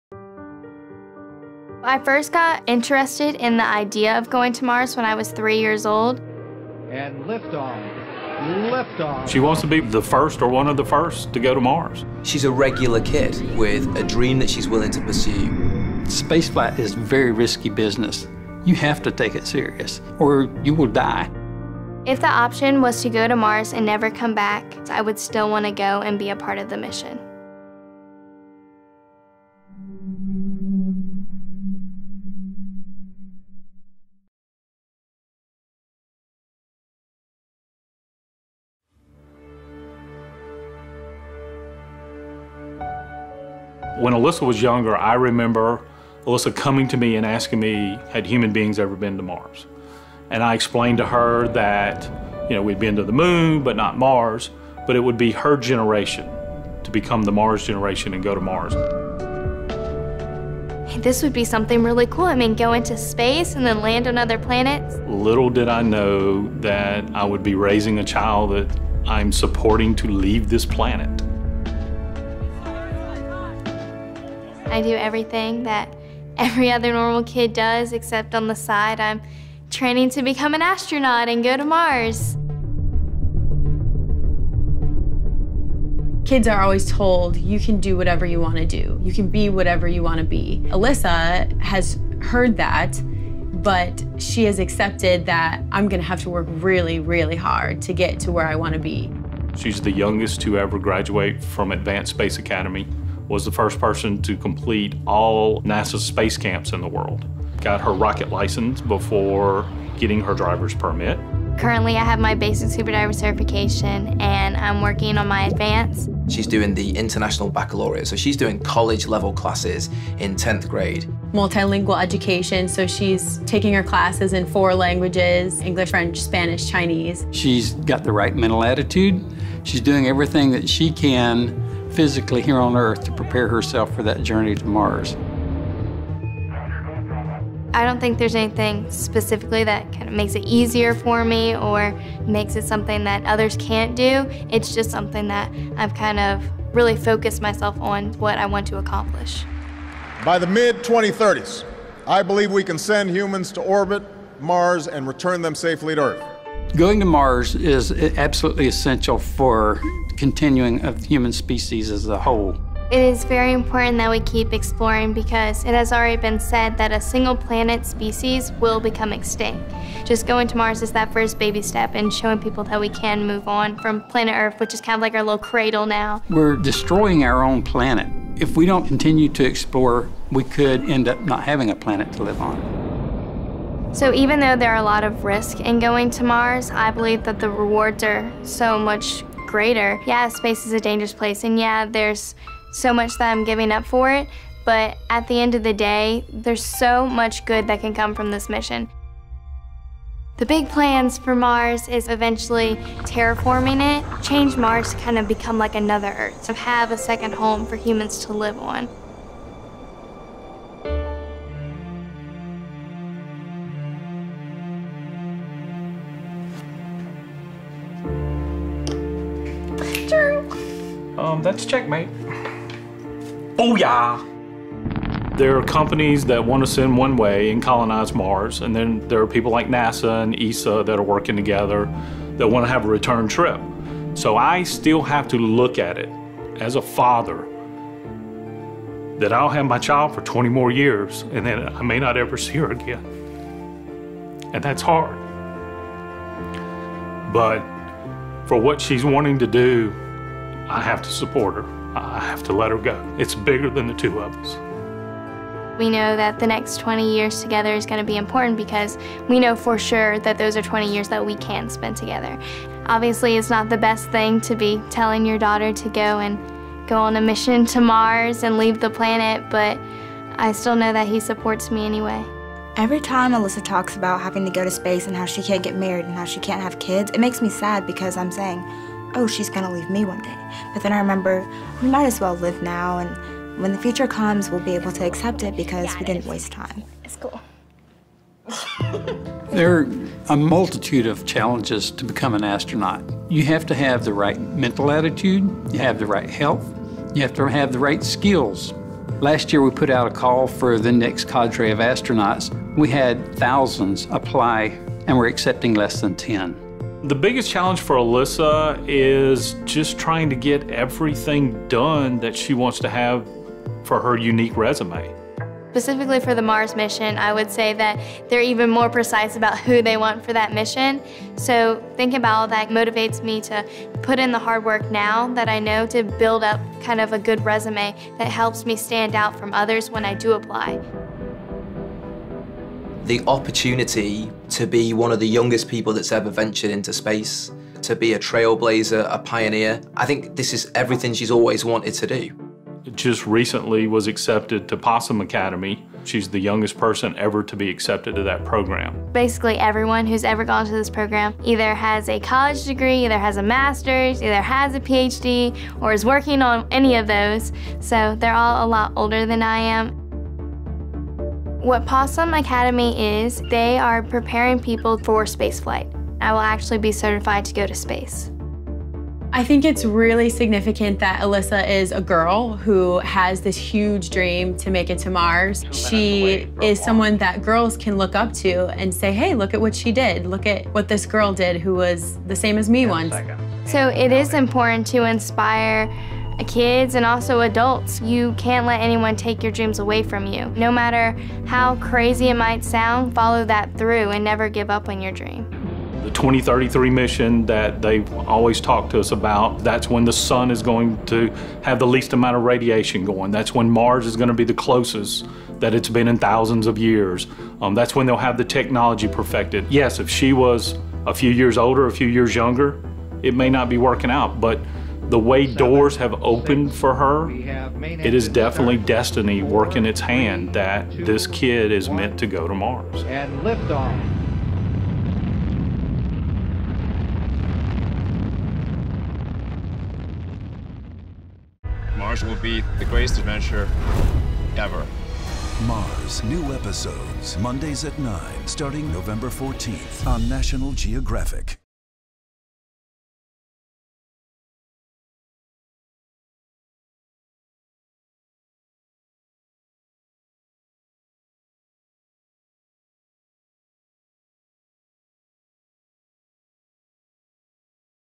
Mission To MARS! 15-Year-Old Alyssa Carson Could Be The First Human On Mars
Tune into the global event series, MARS, Mondays at 9/8c, premiering November 14 on the National Geographic Channel, to see how humanity's first manned mission to the red planet would unfold.  MakeMarsHome.com